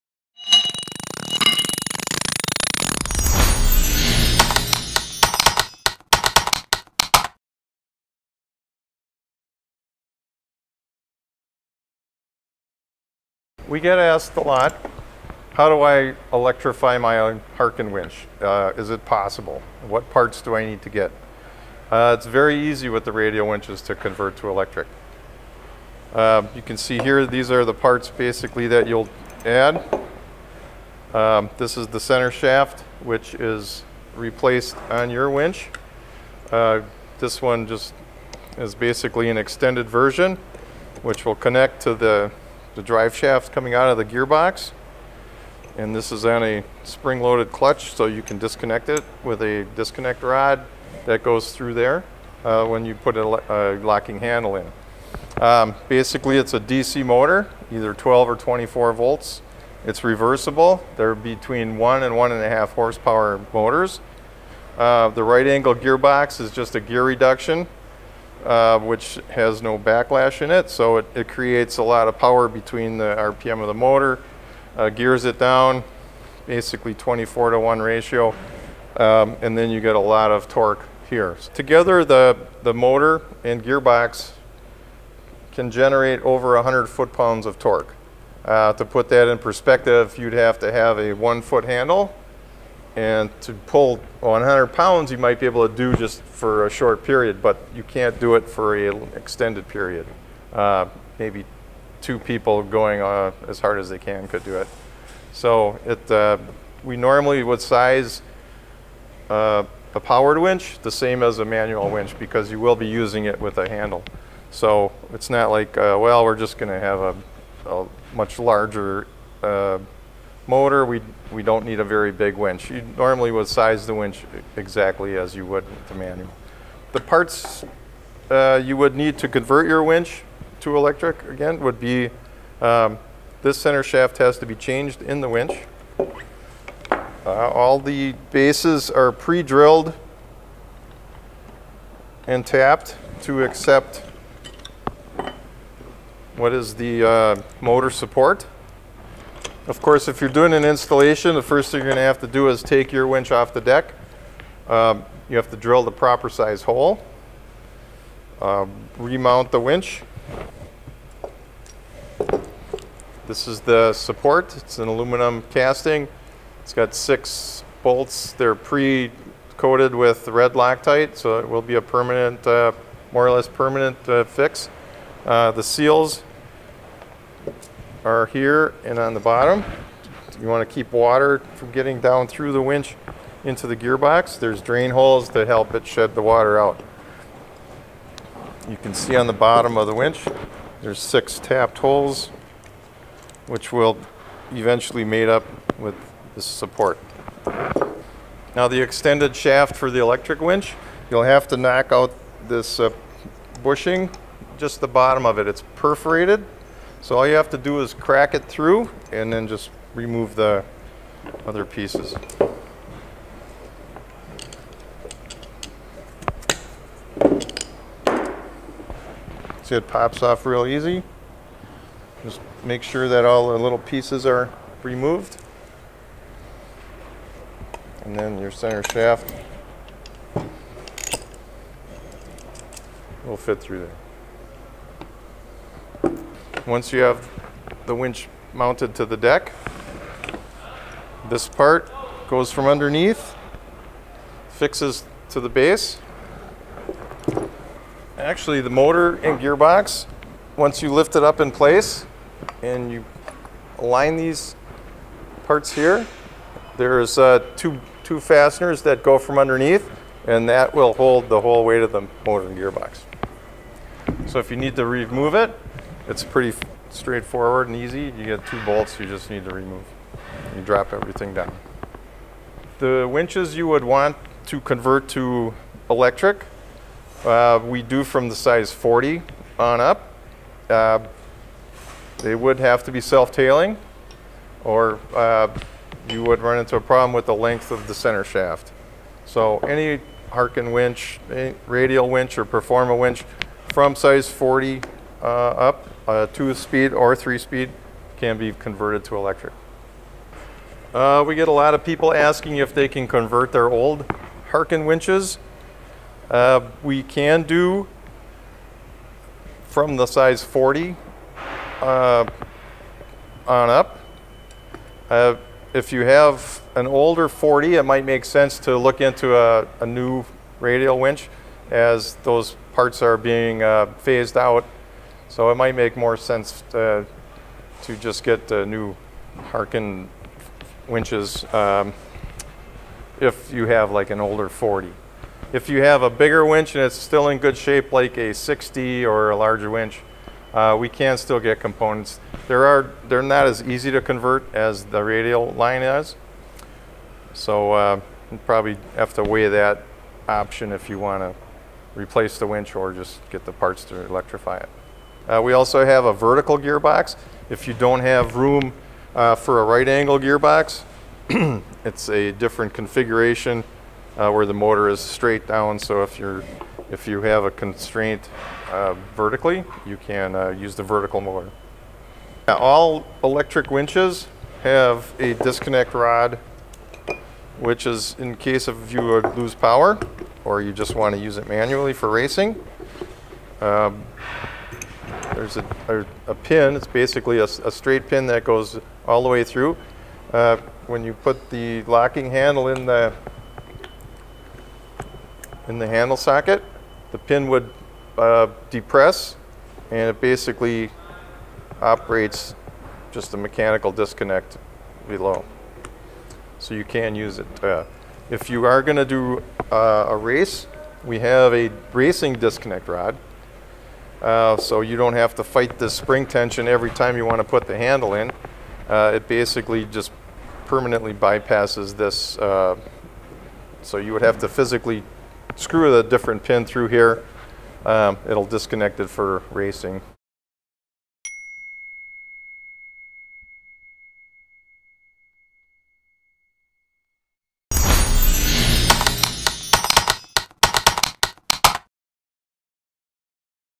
Electric Winch Conversion || Harken Tech Team Asked & Answered
Harken Tech Team member Craig Blazer shows us how easy it is to upgrade your Harken Radial winches to electric power.